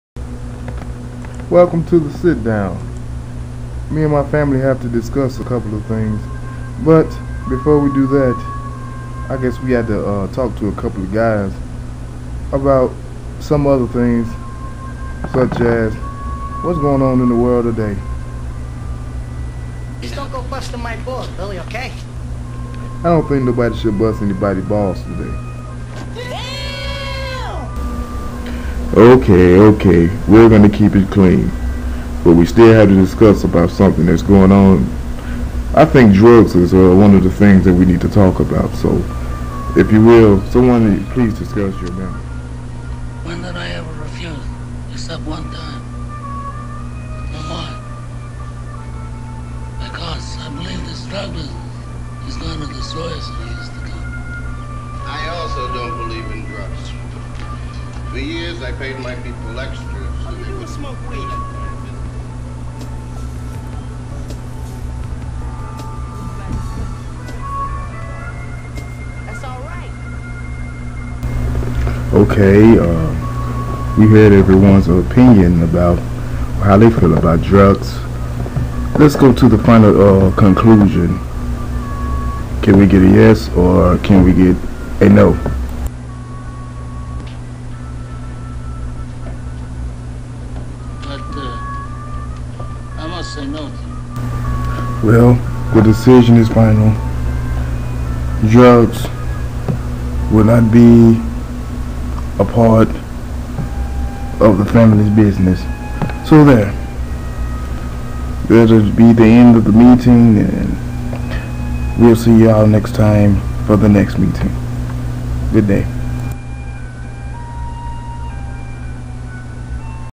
Doodle Don's The Sit Down .wmv
Doodle Don sits down with the family to discuss if drugs should be permitted in the family's business.